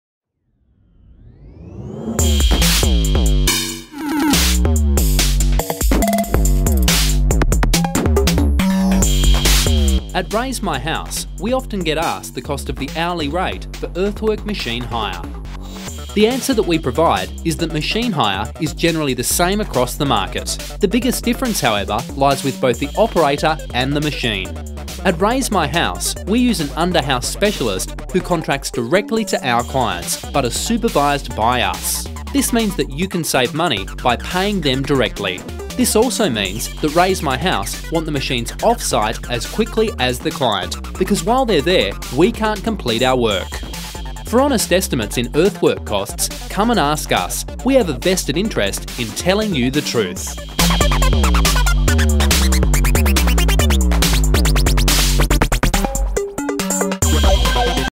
Brisbane Home Renovation and Under House Dig Out
Raise My House have a vested interest in making sure your earthworks take as little time as possible. Hear about how we can help you with your under house dig outs for your next Brisbane renovation project.